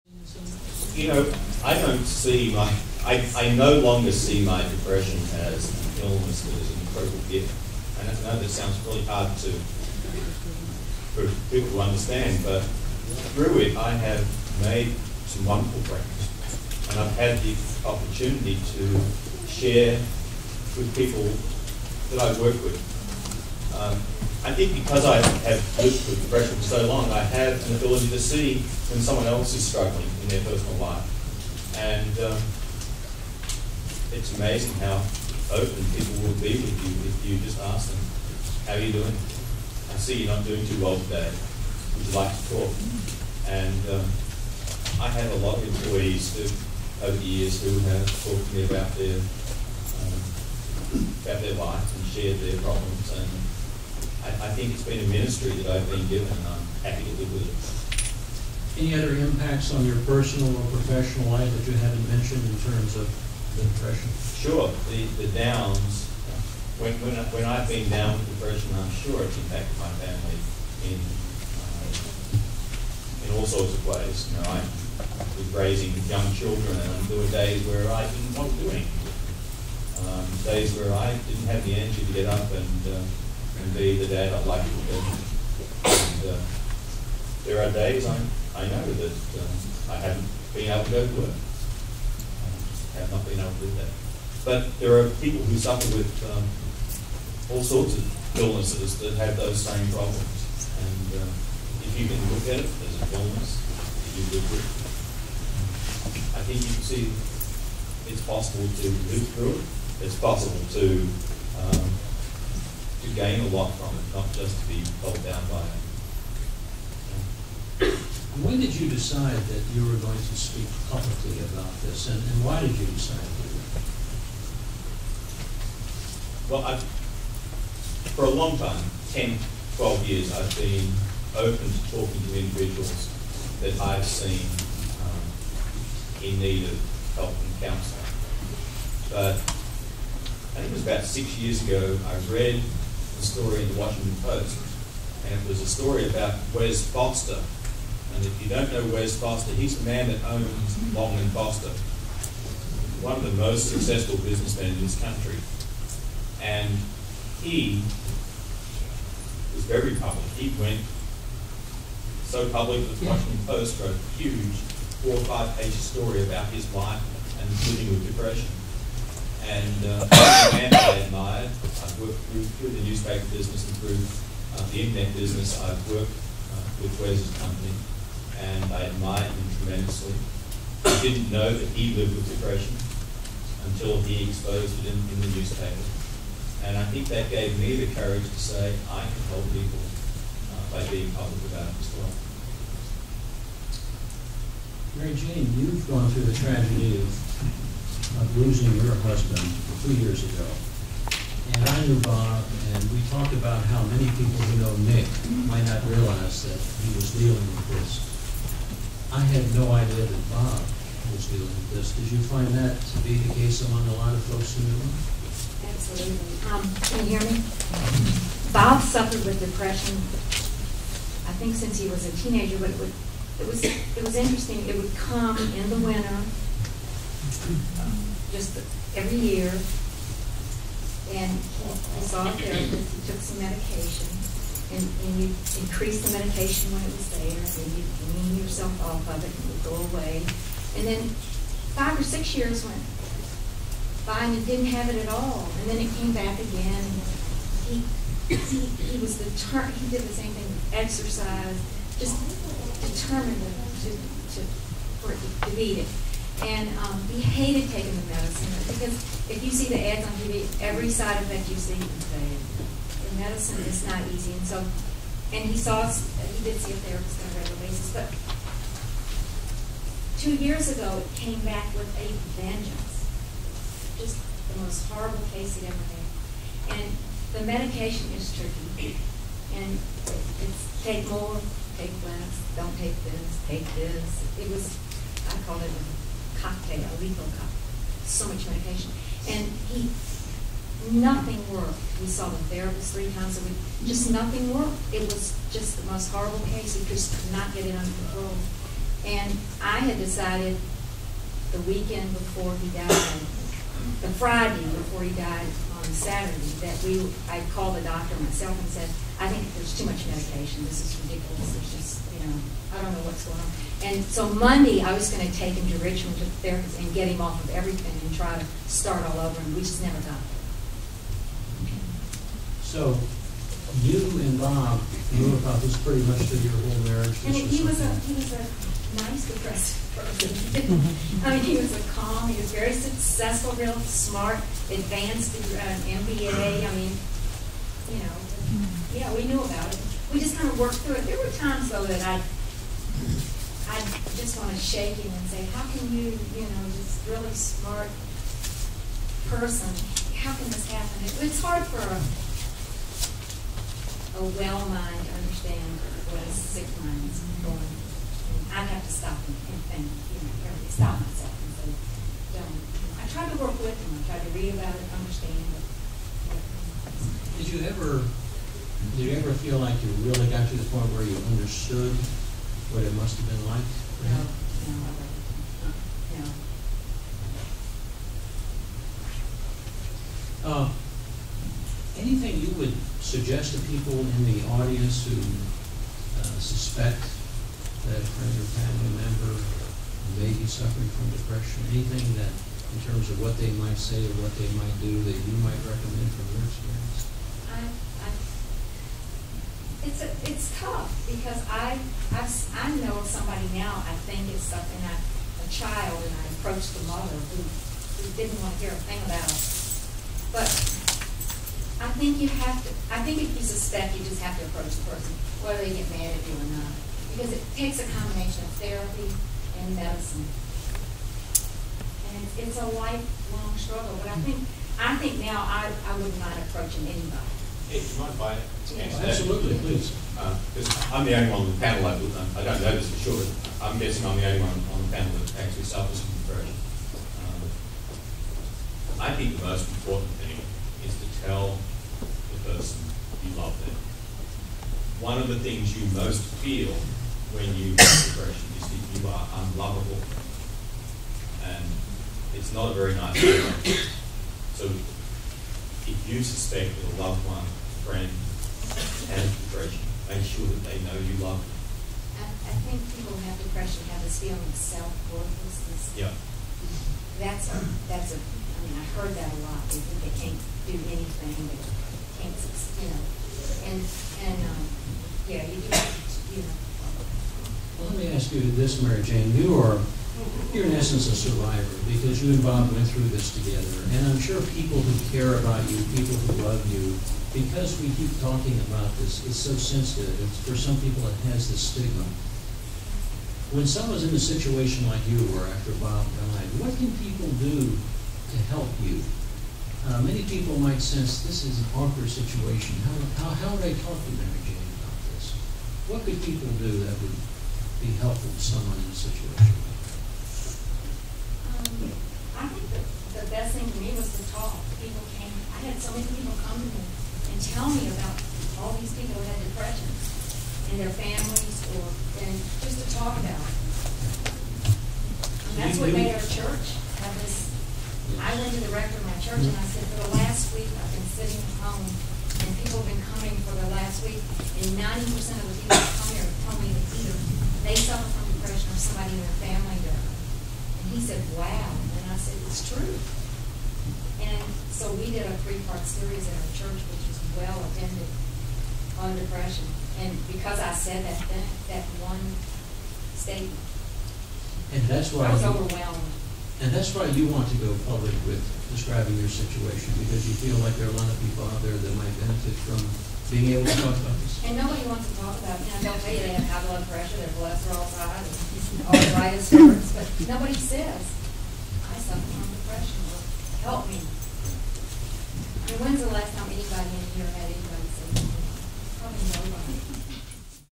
Part 2: GCC panel discussion on depression, bipolarism and suicide
"One of the feelings you have when you suffer from depression is that you are unloveable. Make sure they know you love them"
 GCC Dean Mary Gilkey will teach a 12-hour course on Mental Health First Aid Oct. 22 and Oct. 29, 2011 in Fredericksburg, Va. Call 540/891-0312 to register, and you may save a life.